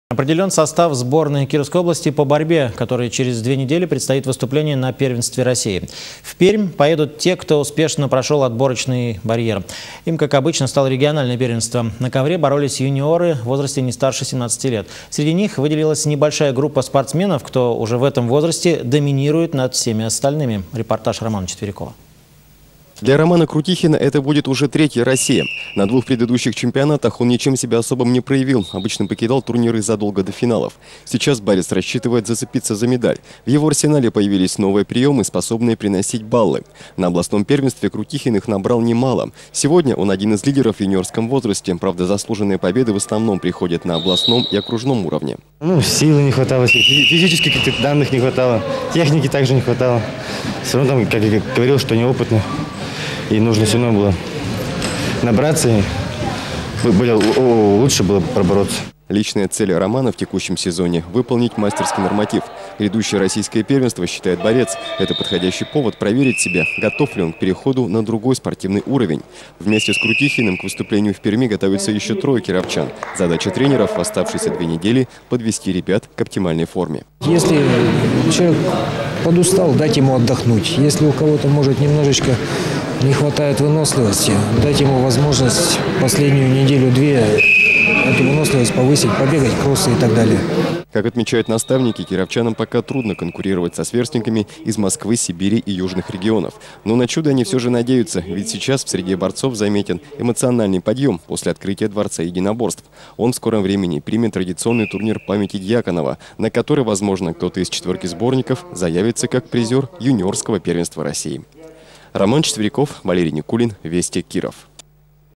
Борьба (ГТРК Вятка)
Определен состав сборной области по борьбе, которой через две недели предстоит выступление на Первенстве России. В Пермь поедут те, кто успешно прошел отборочный барьер. Им, как обычно, стало региональное Первенство. На ковре боролись юниоры не старше 17 лет. Среди них выделалась небольшая группа спортсменов, кто в этом возрасте доминирует над всеми остальными.